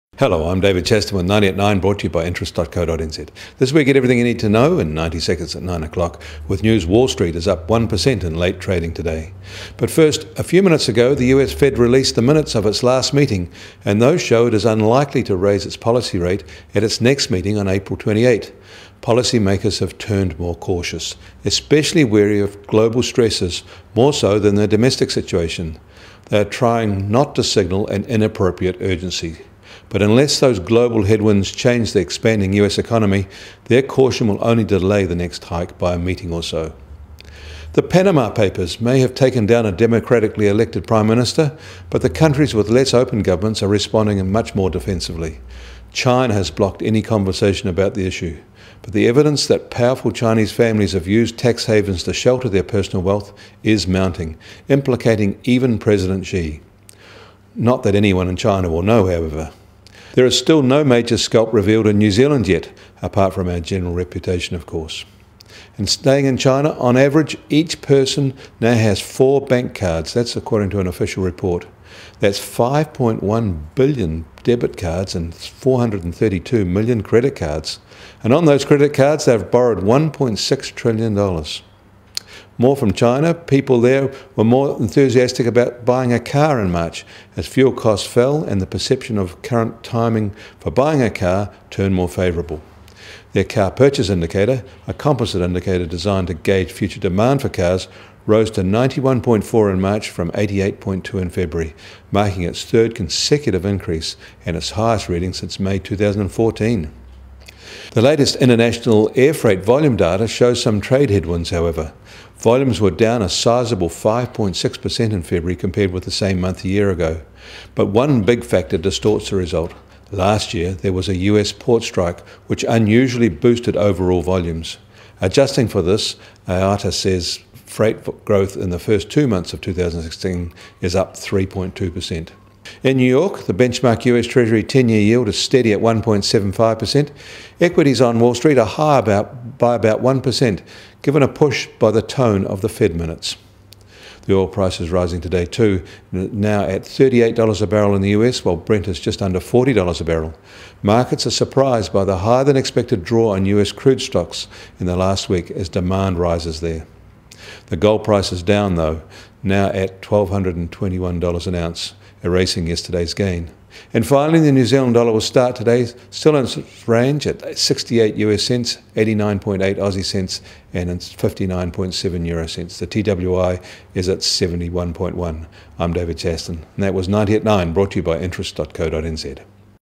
90 seconds @ 9am : Stocks rise on timid Fed
The US Fed released the minutes of its last meeting and those show it is unlikely to raise its policy rate at its next meeting on April 28. Policy makers have turned more cautious, especially wary of global stresses more so than their domestic situation. They are trying not to signal an inappropriate urgency.

The Panama Papers may have taken down a democratically elected prime minister, but countries with less open governments are responding much more defensively.